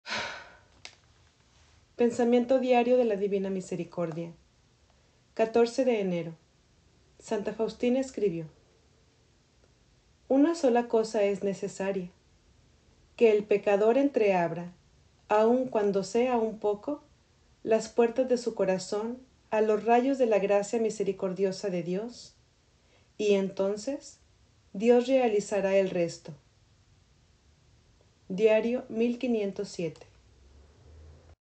14 de Enero. Pensamientos diarios de la Divina Misericordia.
Diálogos entre Jesús y Santa Faustina Kowalska tomados del libro Bajo La Sombra de Tus Rayos. Del Diario de Santa Maria Faustina Kowalska.